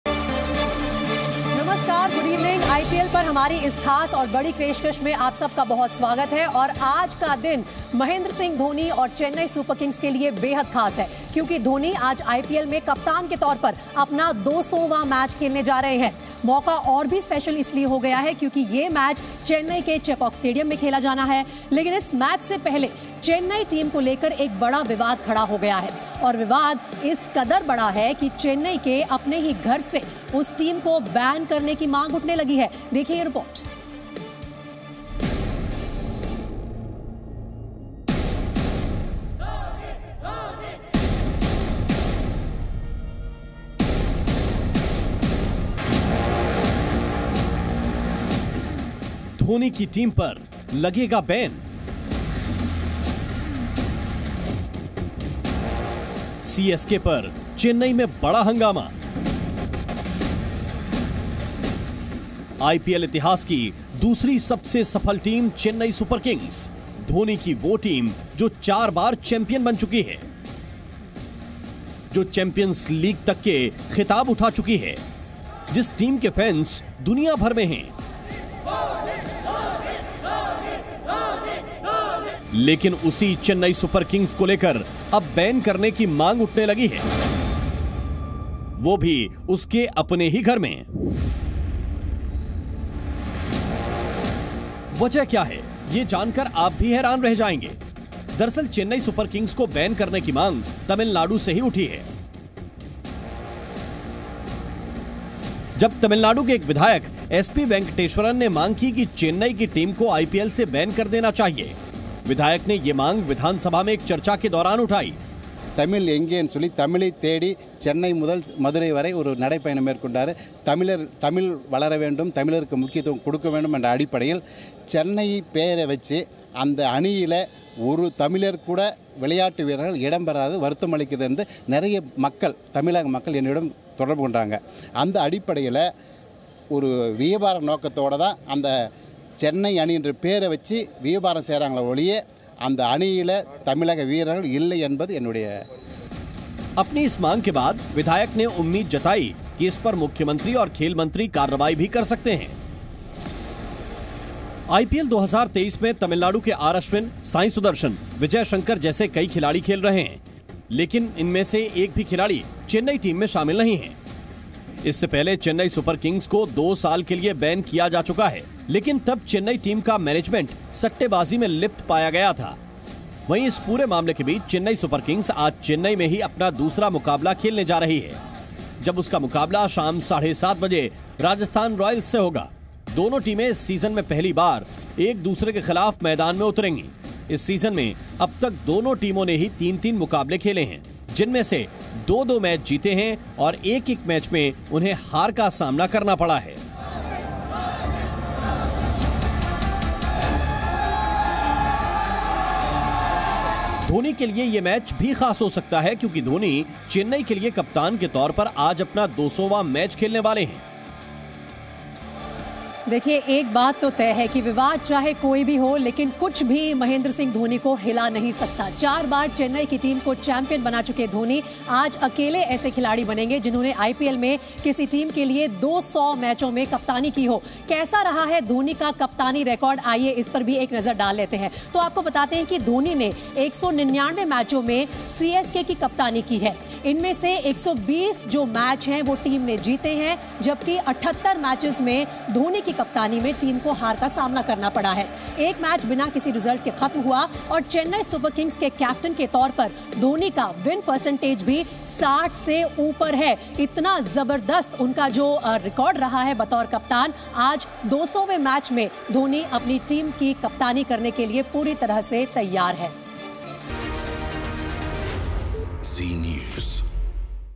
Watch Video: तमिलनाडु विधानसभा में उठी MS Dhoni की टीम CSK को बैन करने की मांग | IPL Highlights 2023
महेंद्र सिंह धोनी का आज CSK के कप्तान के रूप में 200 वां मैच है. लेकिन इस ऐतिहासिक मैच वाले दिन CSK को बैन करने की मांग उठी है. तमिलनाडु विधानसभा में CSK को बैन करने की मांग की गई है.

Today is Mahendra Singh Dhoni's 200th match as the captain of CSK. But on the day of this historic match, there has been a demand to ban CSK. There has been a demand to ban CSK in the Tamil Nadu Assembly.